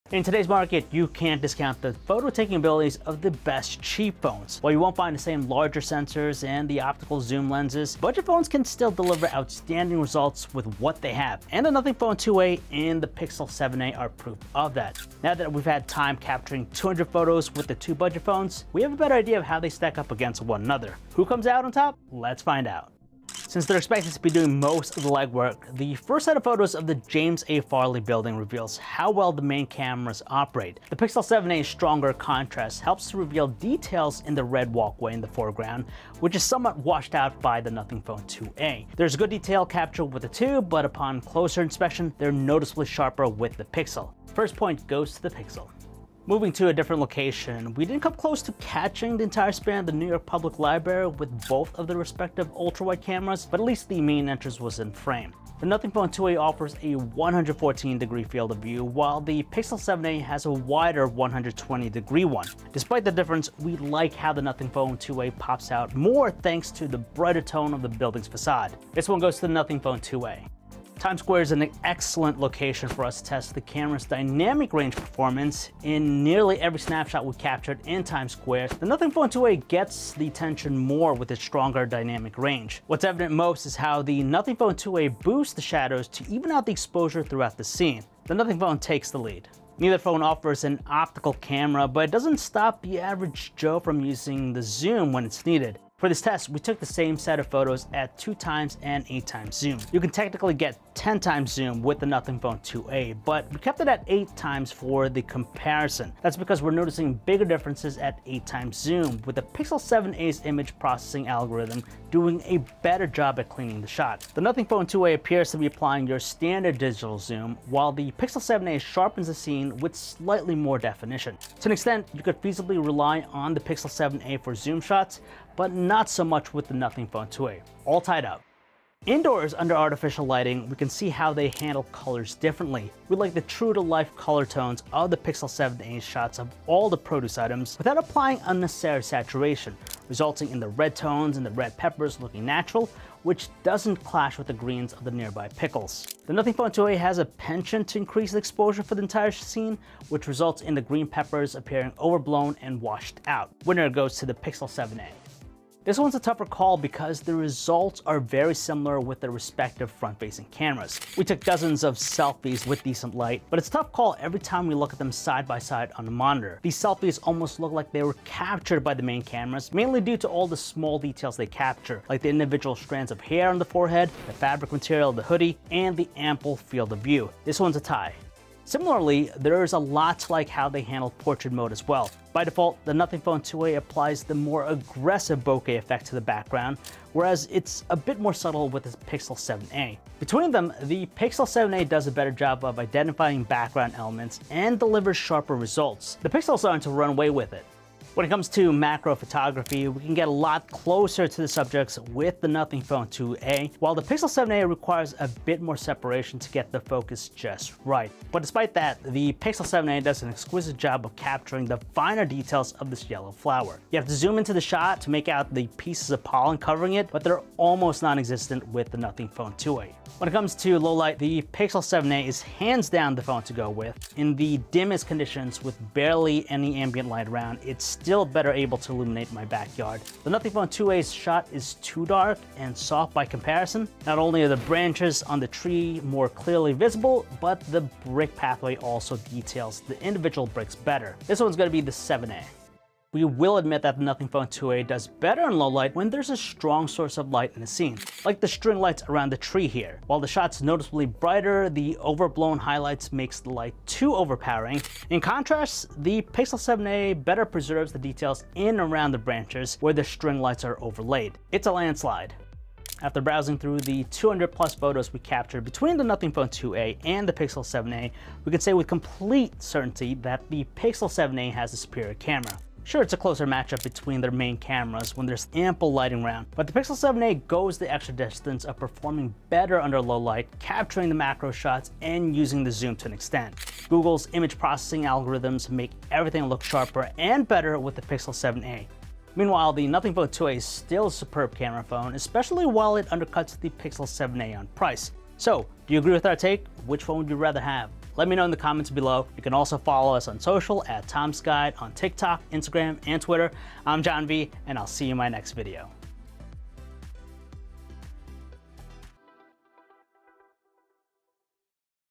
We Took 200 Photos With The Google Pixel 7a vs Nothing Phone (2a) | Photo Shoot|out
We compared the Google Pixel 7a vs Nothing Phone (2a) through a 200 photo face-off to decide which is the best budget smartphone for photography. Google’s computing and Nothing's larger camera sensor shine. There are also image processing algorithms that both phones apply to photos, which tweaks them in such a way to deliver even better results. But, there can only be one winner.
This is Photo Shoot|out!

Our favorite deals:
MacBooks on sale